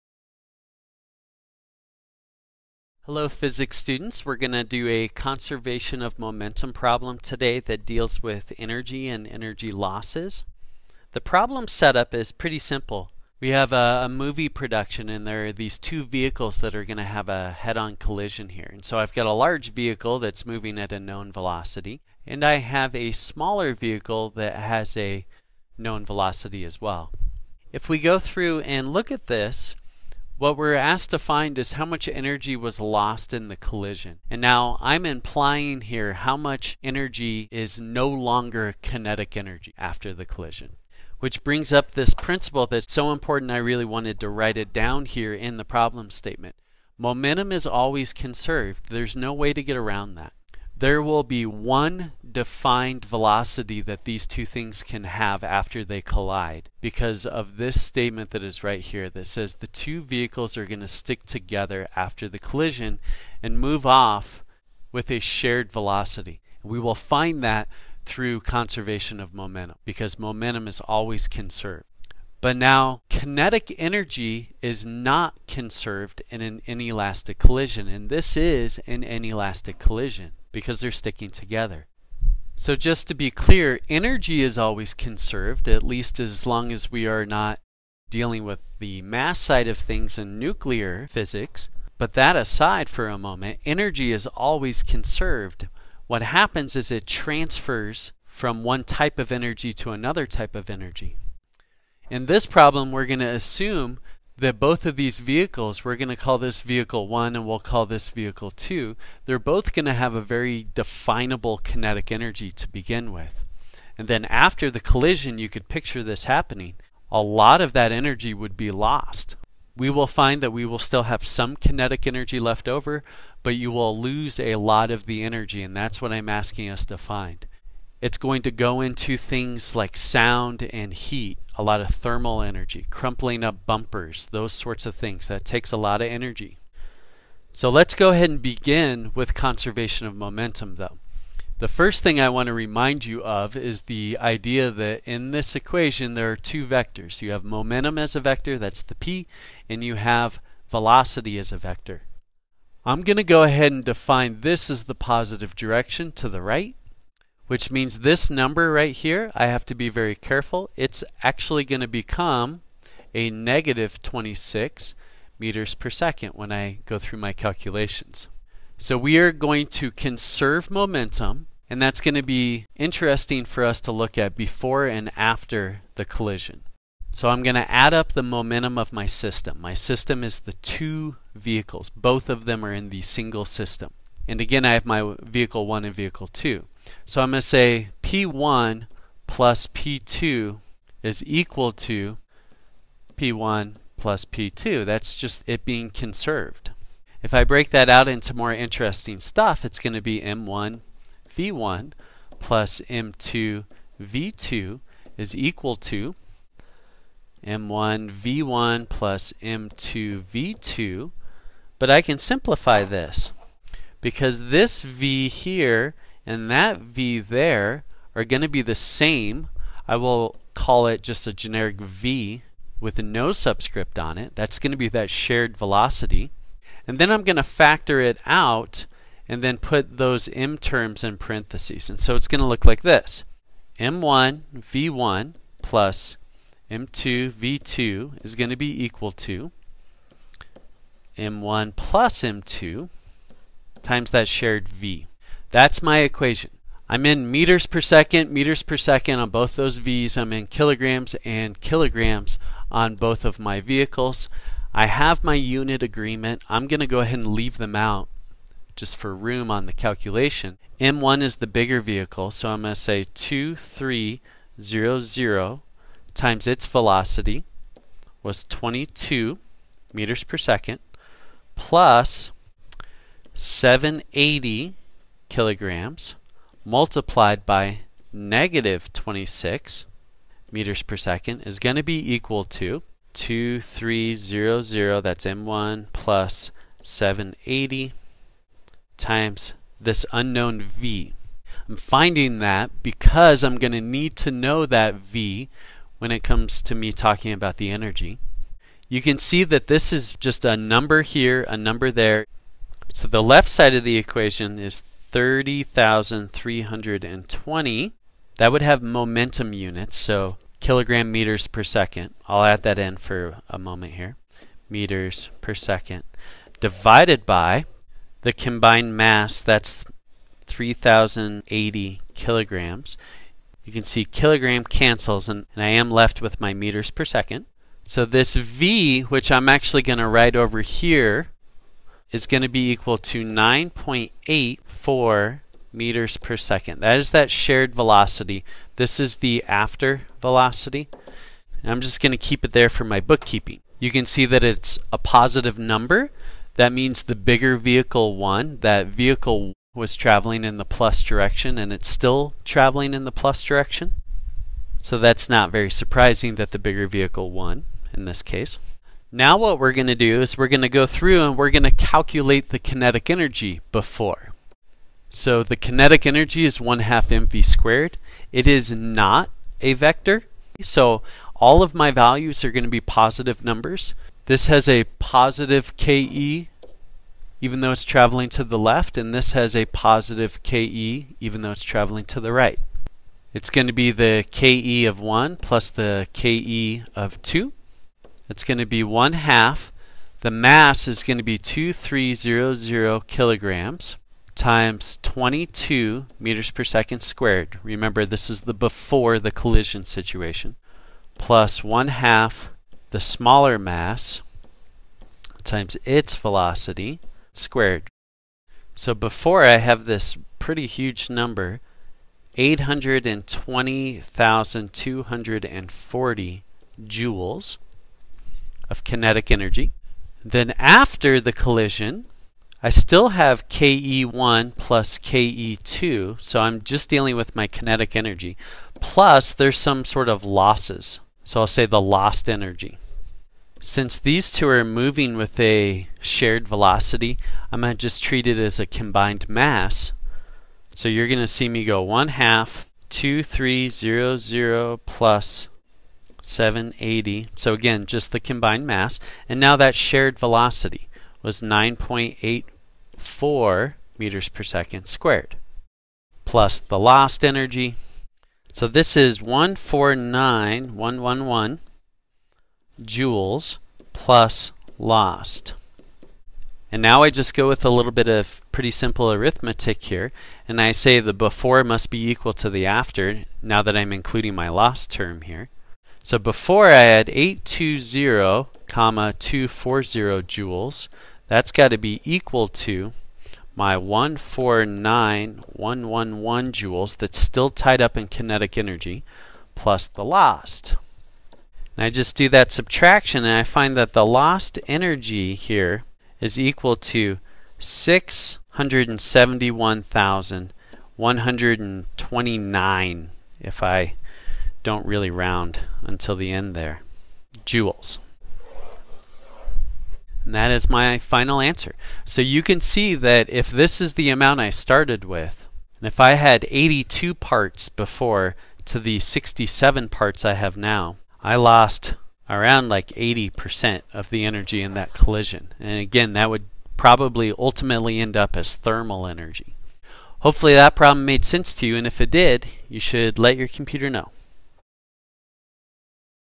Energy Lost in an Inelastic Collision: Sample Physics Problem
This video demonstrates calculations using conservation of momentum and kinetic energy for an inelastic collision.  for more!